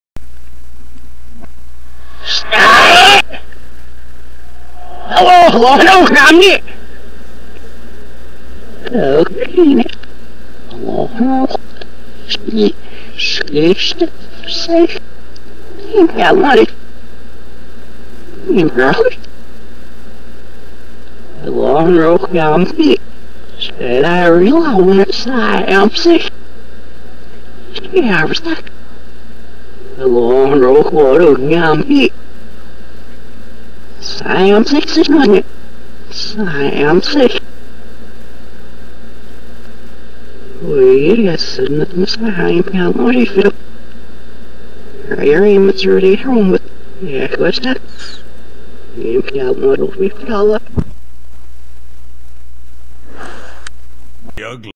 Chris Crocker- Eat My Cornhole Reversed + Subliminal Message
Title explains all, i dont think this is as gay as my first two videos but it might be lol. i discovered the subliminal message trying to entertain myself by reversing youtube videos. enjoy